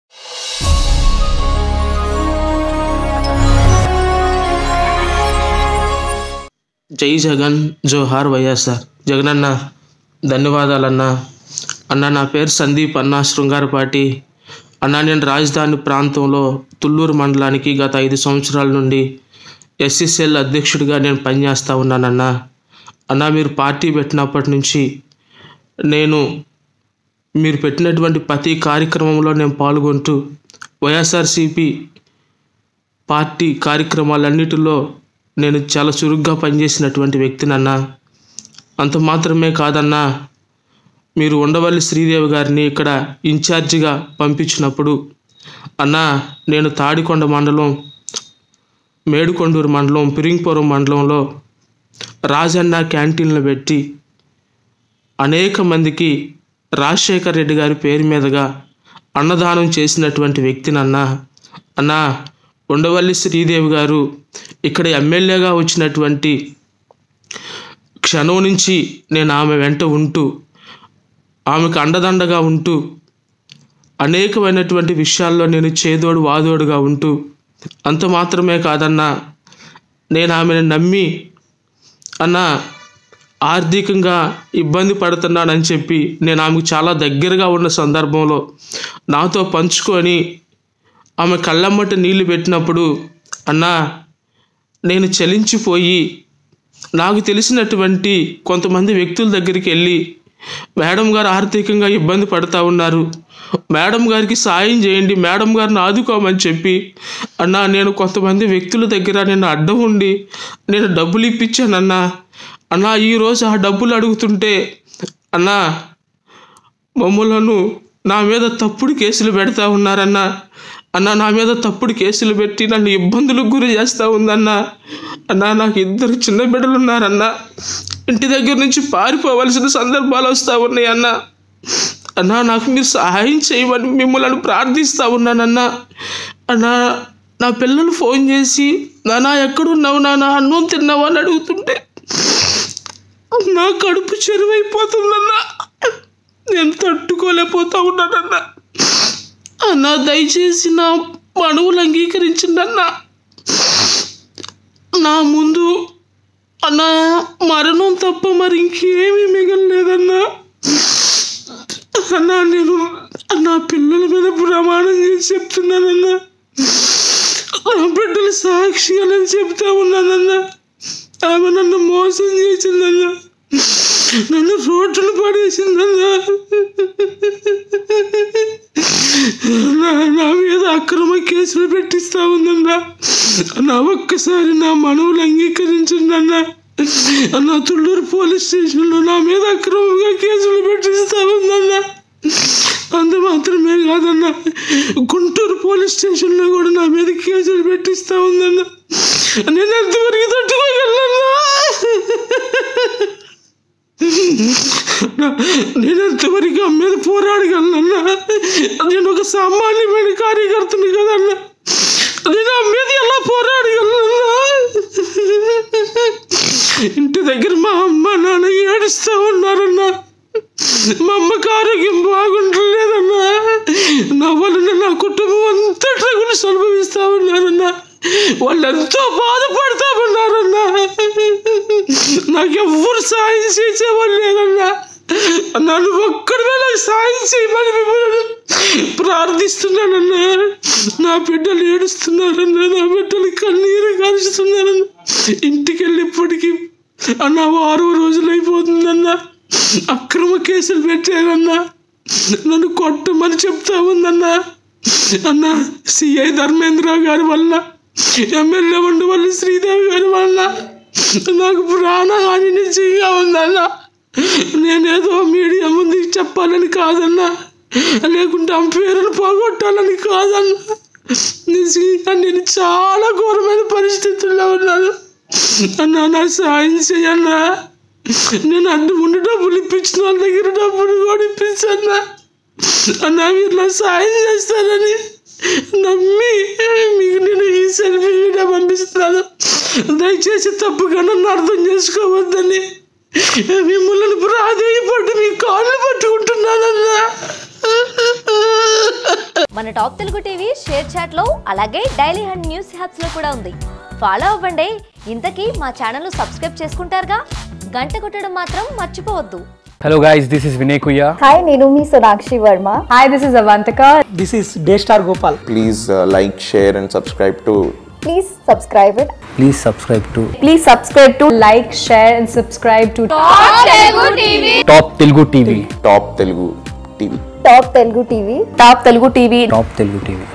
Ycp Activist Shocking Allegations on YSRCP MLA Vundavalli Sridevi | YS JAGAN | Ap Political News
Ycp Activist Shocking Allegations on YSRCP MLA Vundavalli Sridevi | YS JAGAN | Ap Political News in Telugu | Top Telugu TV